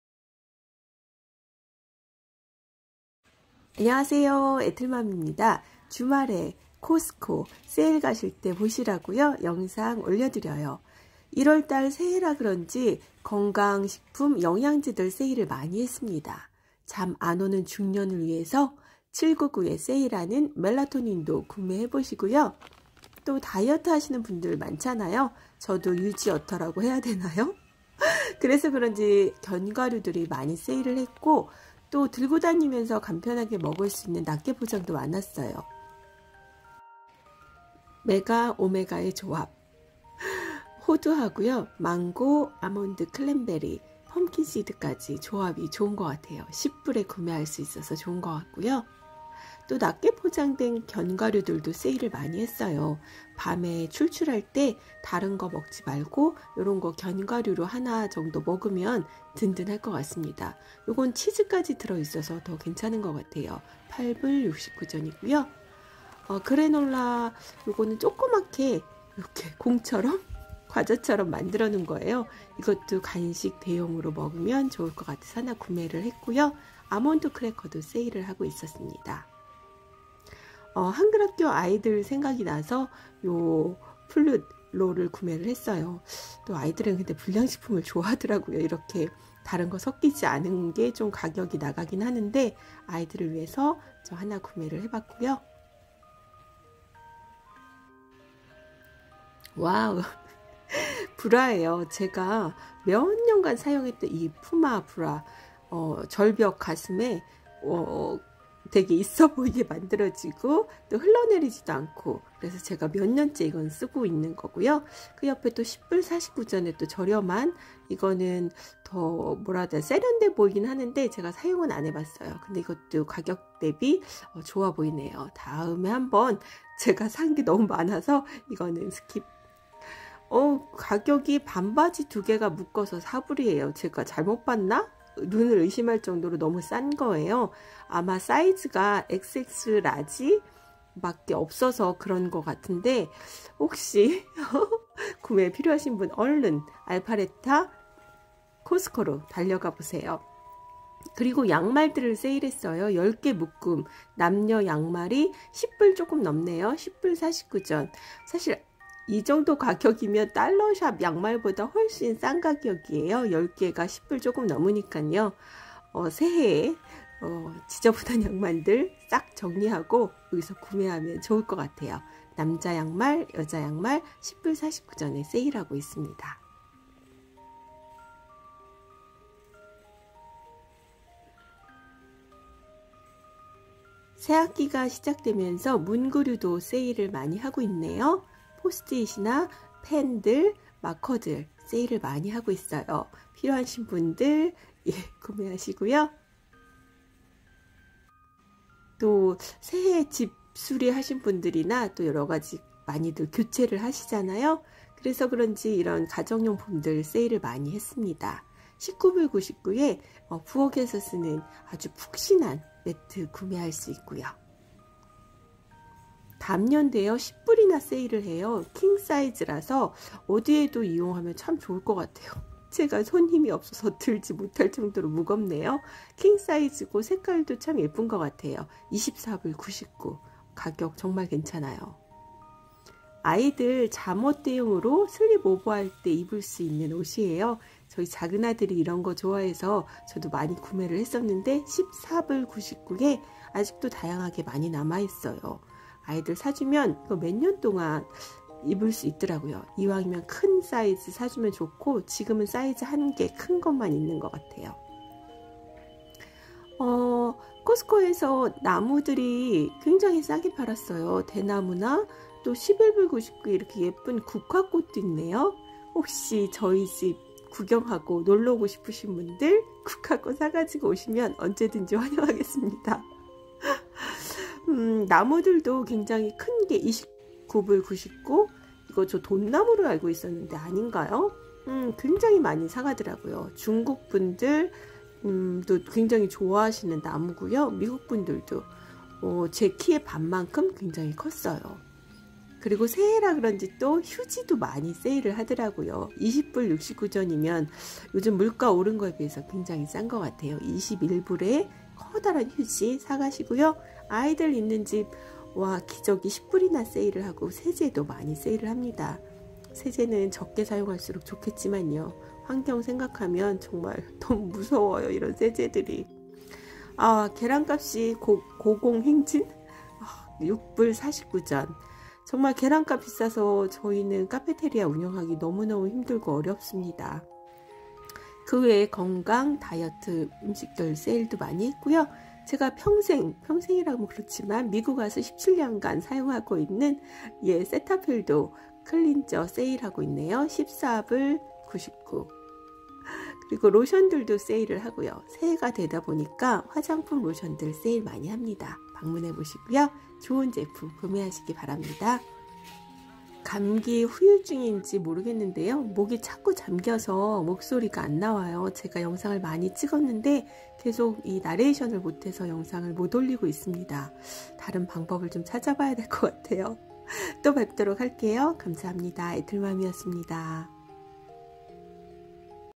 미국 코스트코 추천, 세일 상품 / 20개 추천 아이템 / 애틀랜타 알파레타 코스코 Costco Shopping
안녕하세요.
미국 조지아 애틀랜타에 사는 애틀맘입니다.
1월 코스트코 세일 추천 쇼핑 영상입니다.
생활에 꼭 필요한 제품중 세일하는 제품으로 모았습니다.
오늘 영상이 쇼핑에 도움이 되면 좋겠습니다.
감사합니다. 건강하세요.

*미국에서 한국 방송 스트리밍 전문 서비스인
온디멘드코리아 데일리 라이프에서도 애틀맘 영상을 보실 수 있습니다.